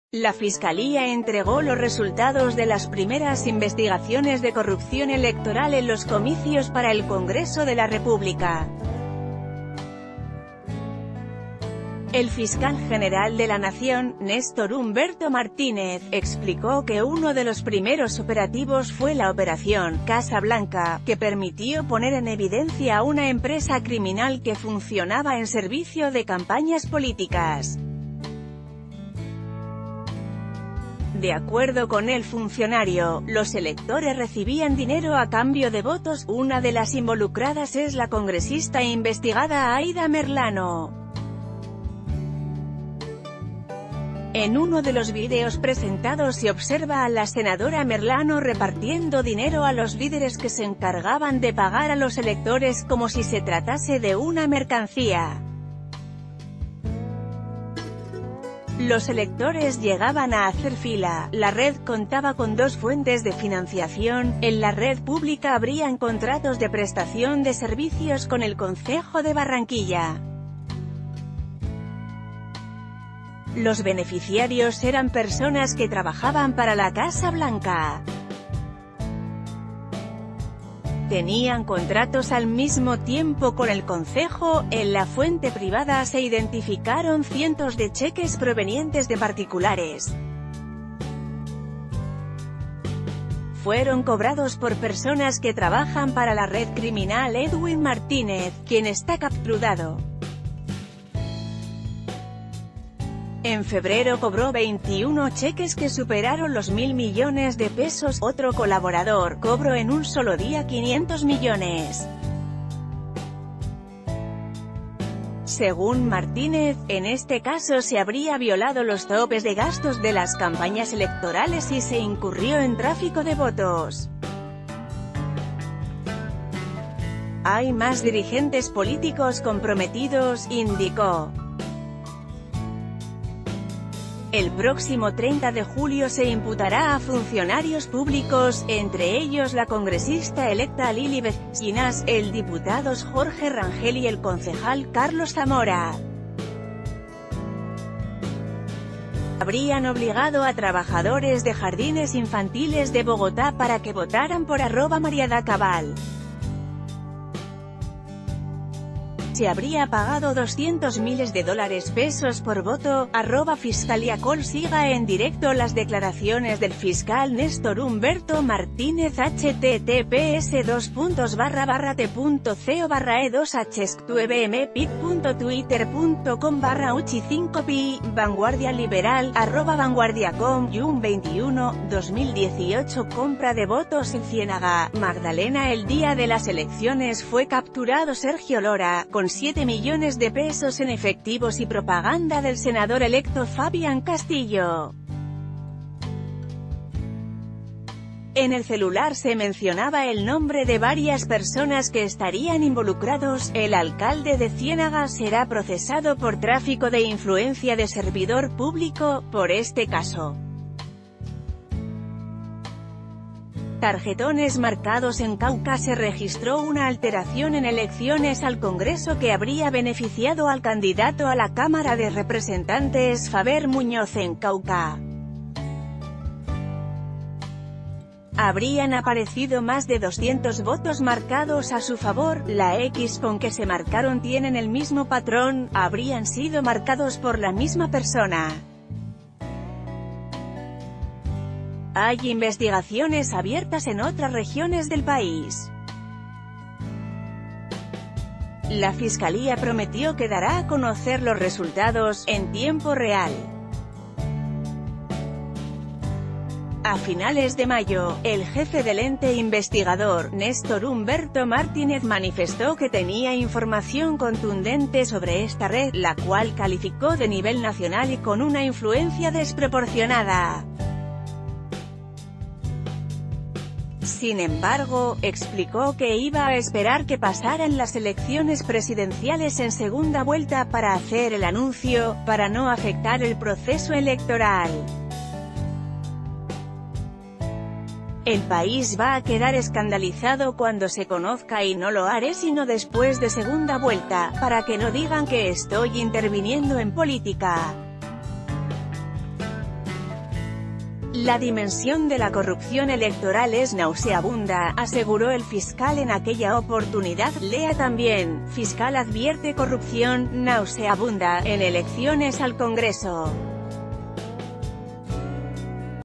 Estas son las revelaciones de la Fiscalía sobre la red nacional de corrupción electoral Noticias ...
Estas son las revelaciones de la Fiscalía sobre la red nacional de corrupción electoral Noticias 24/7
Noticias 24/7